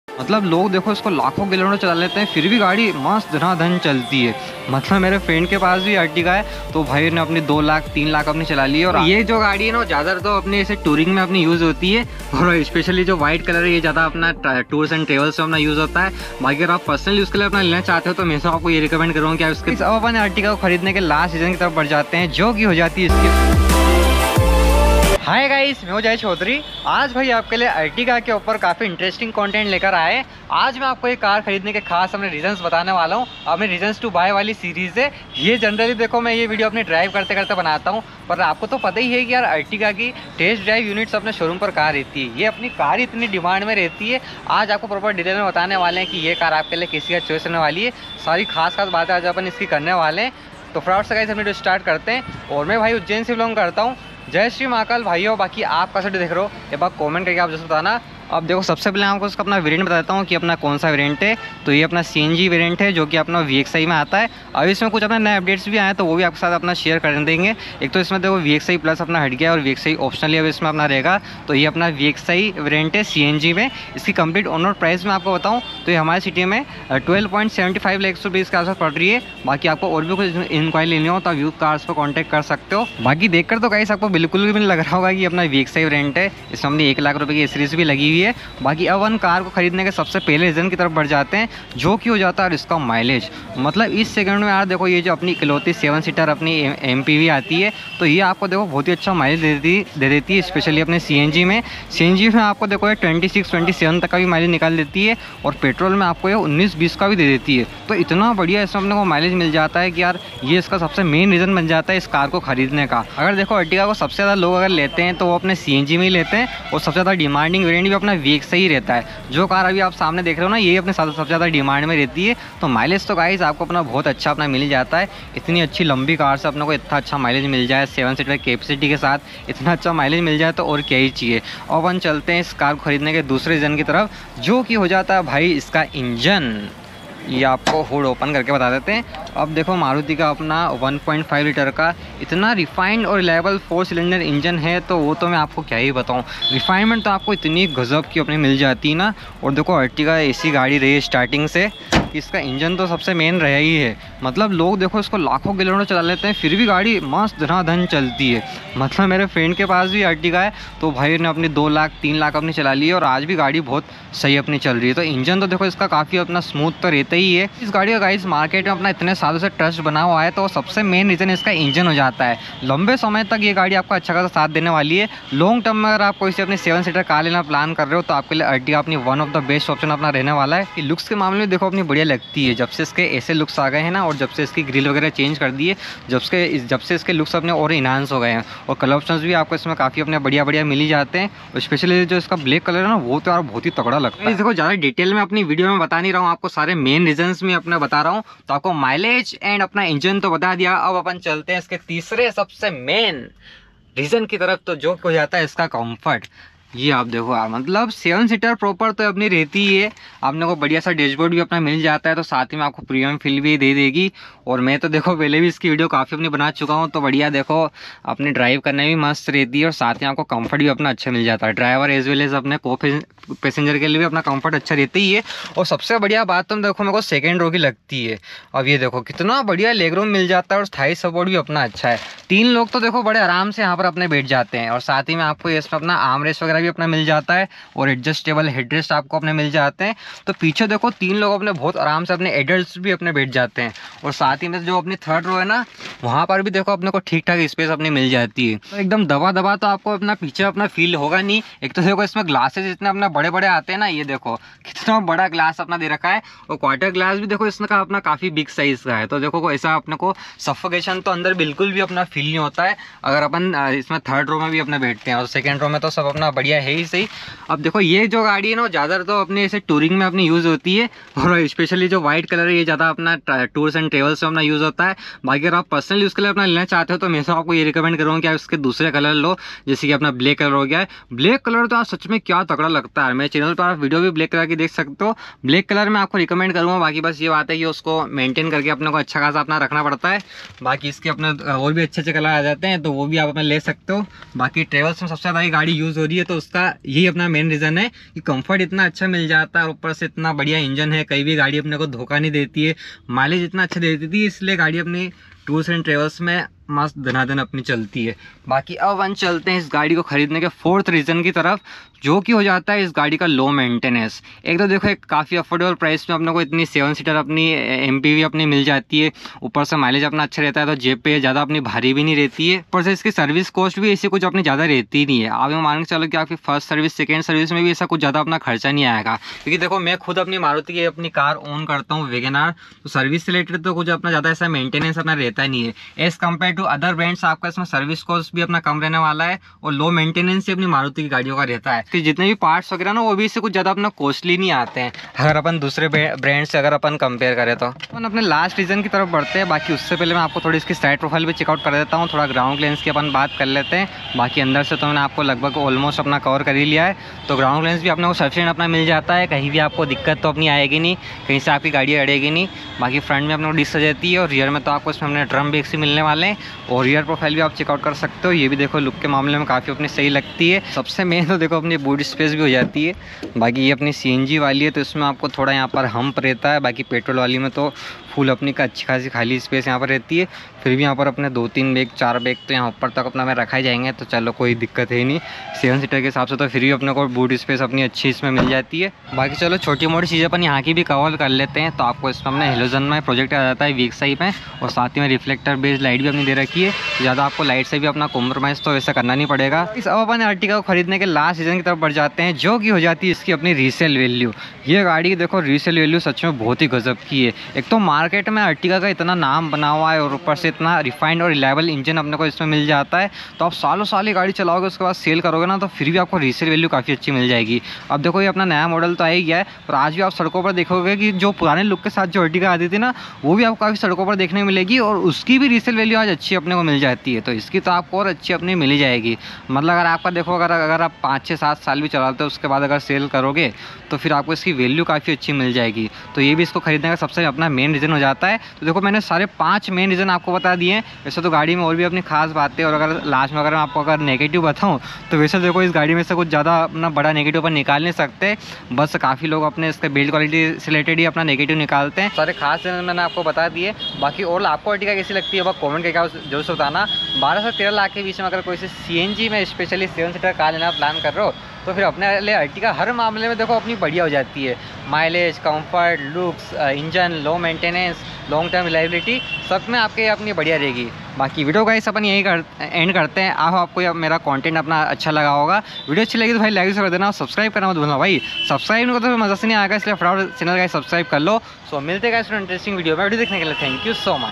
Should You Buy New Ertiga In 2024? | Reasons To Buy Maruti Suzuki Ertiga Vxi CNG
Keep Supporting💛